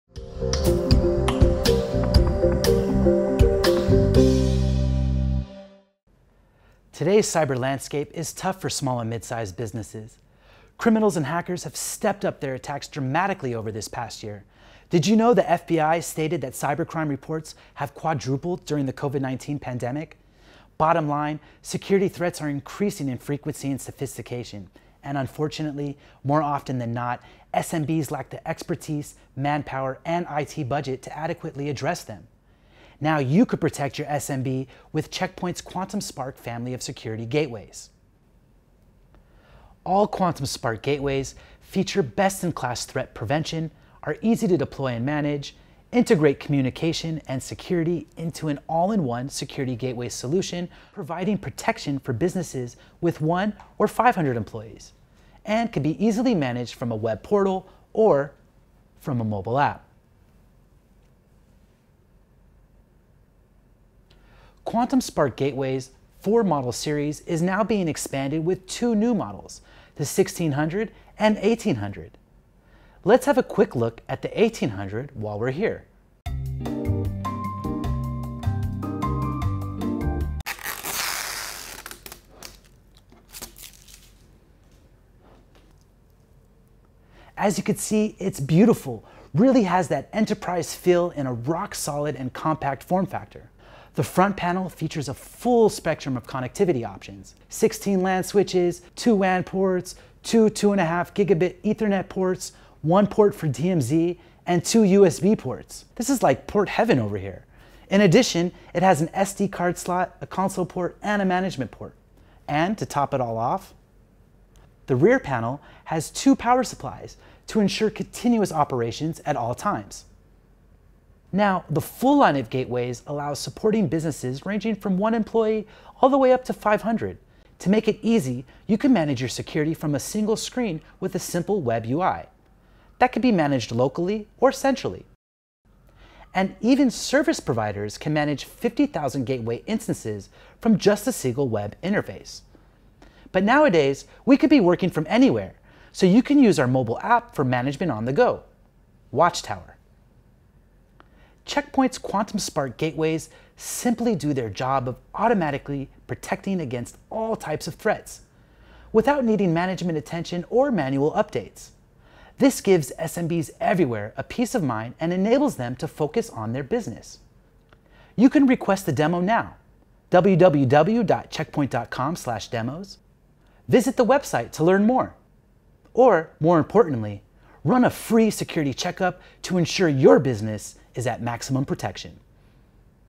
Quantum Spark: Advanced Protection for Small and Medium Businesses
Protect your SMB with new Quantum Spark™. The Quantum Spark Next Generation Firewalls for small and medium size businesses, feature best-in-class threat protection, are easy to deploy and manage, and integrate communication and security into an “all in one” security gateway solution. Quantum Spark security gateways provide protection for businesses with one to 500 employees, and can be easily managed from a web portal and from a mobile app.